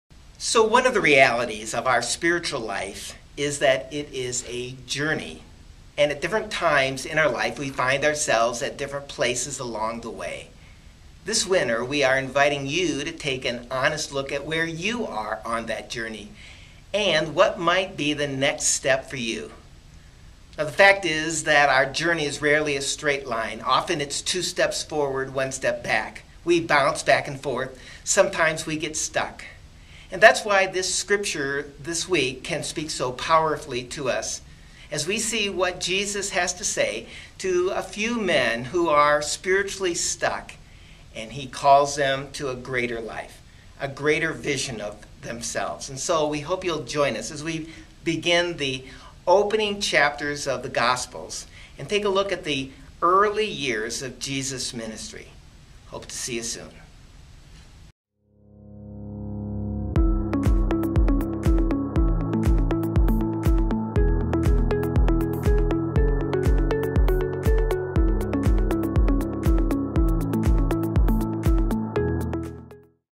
Sermon Preview January 27th & 28th
Paul's Preview - Week 5 of the "Encountering Jesus" series - The Beginning Ministry of Jesus - Going along with the Essential Jesus Reading Challenge - January 27th & 28th, 2018 - Family in Christ Community Church